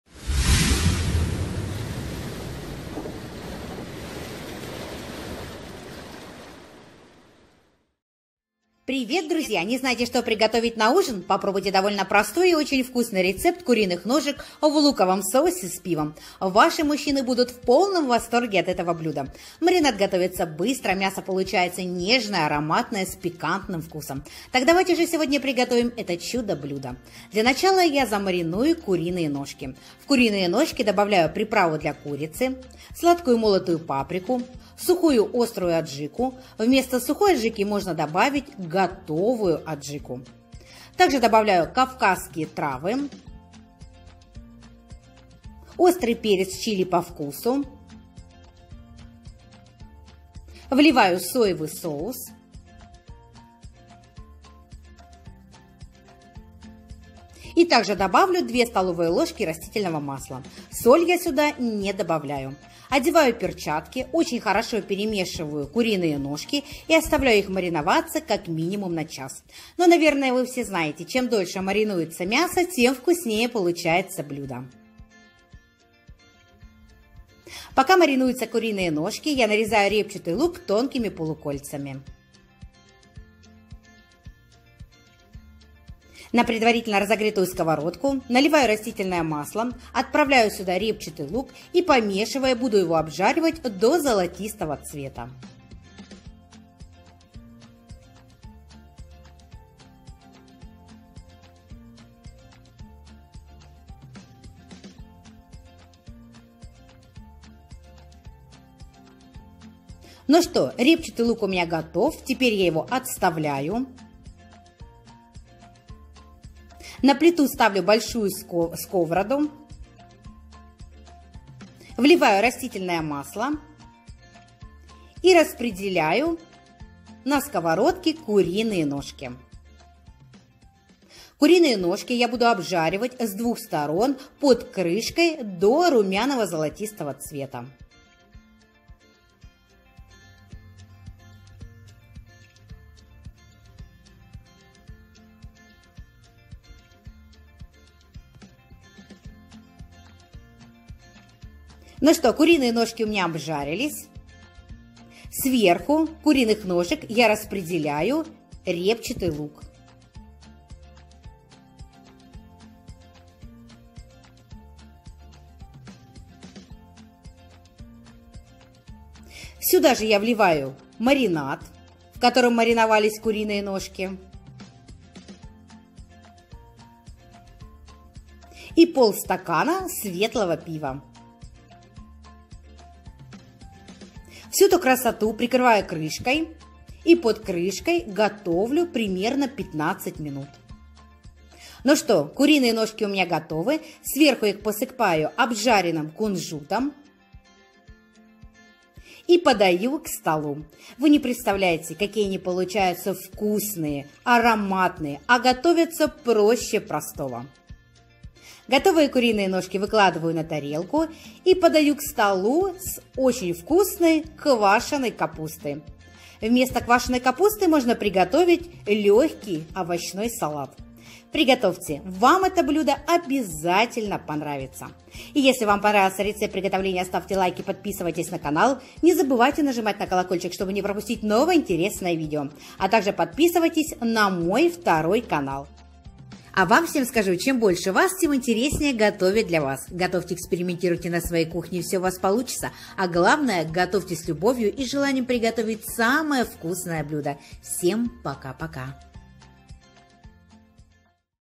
Готовлю так уже 20 ЛЕТ! ВКУСНЕЕ Я ЕЩЕ НЕ ЕЛА! Вместо надоевших КОТЛЕТ!
Нам потребуется:
куриные ножки 6-7 шт
лук репчатый 1 шт
соевый соус 100мл
паприка сладкая 1/2 ч.л
растительное масло 2-3 ст.л
аджика сухая 1 ст.л или готовая 3 ст.л
приправа для курицы 1 ч.л
кавказские травы 1 ч.л
кунжут